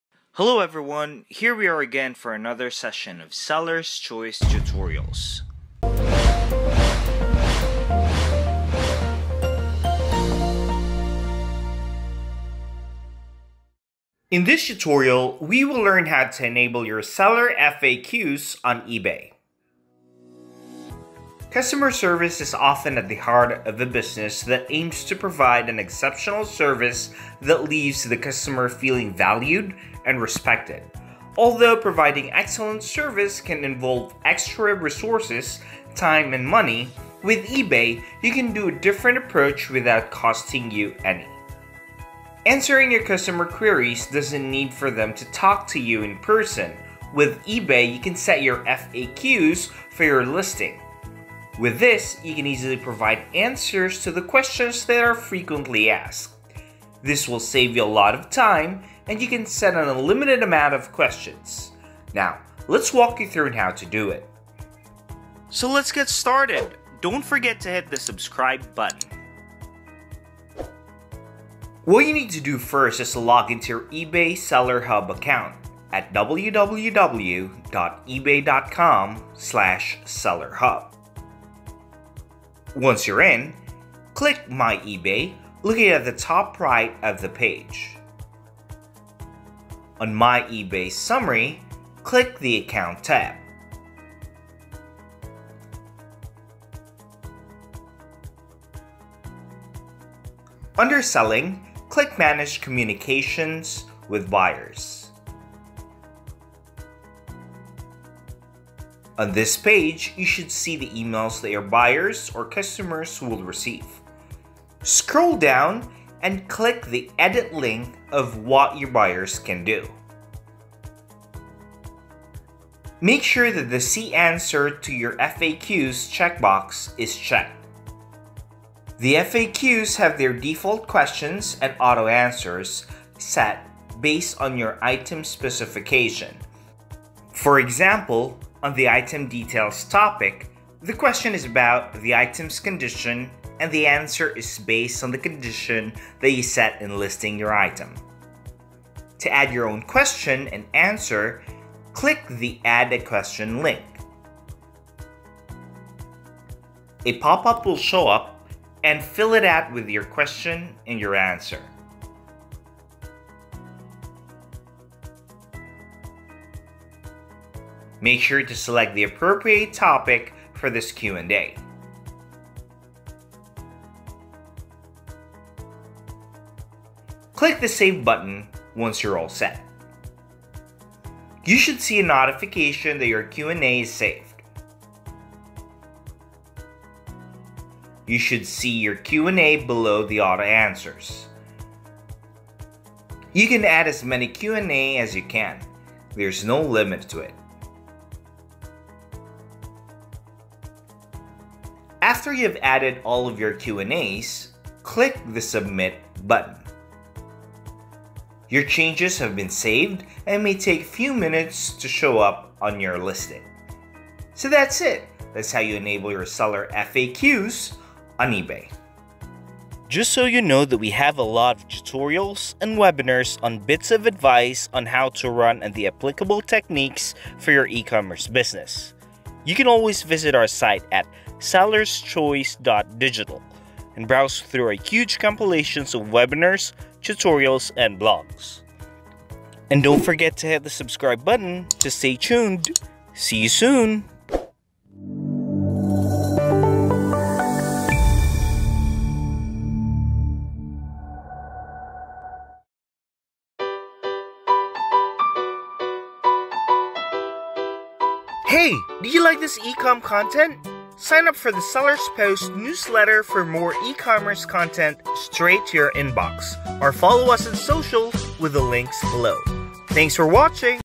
How to Enable your Seller FAQs on eBay - E-commerce Tutorials
Customer service is often at the heart of a business that aims to provide an exceptional service that leaves the customer feeling valued and respected. Although providing excellent service can involve extra resources, time and money, with eBay, you can do a different approach without costing you any.

Answering your customer queries doesn’t need for them to talk to you in person. With eBay, you can set your FAQs for your listing. With this, you can easily provide answers to the questions that are frequently asked. This will save you a lot of time and you can set an unlimited amount of questions.

Now let’s walk you through on how to do it.